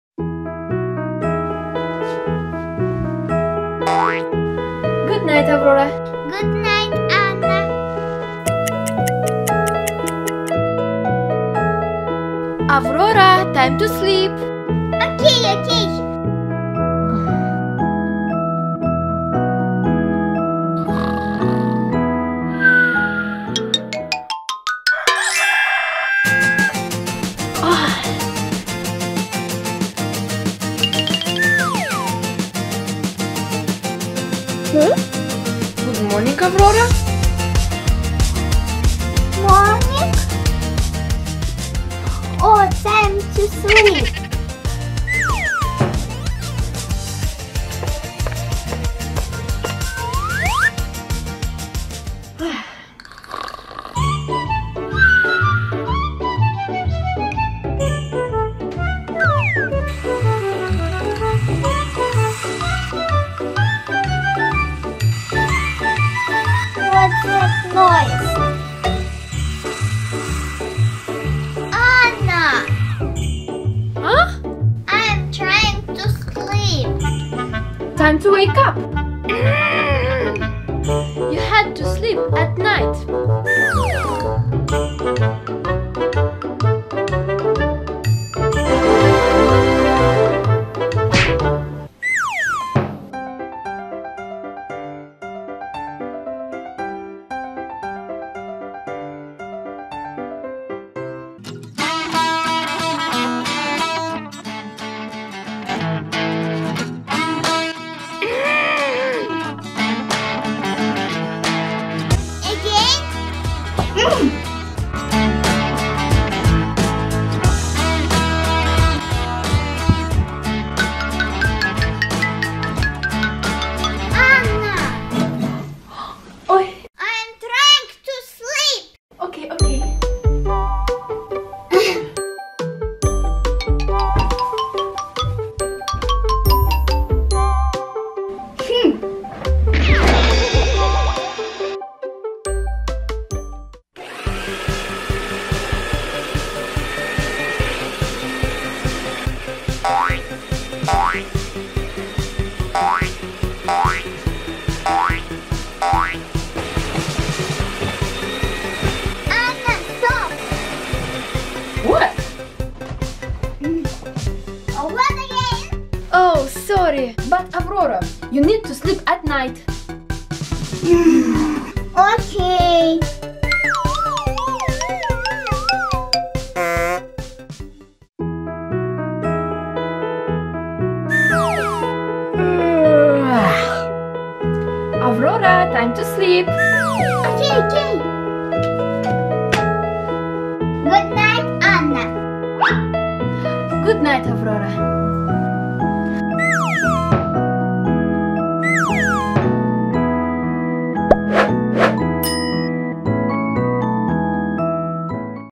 Aurora princess can't wake up!
Elsa as Aurora pretend play with her sister! Best funny video for kids!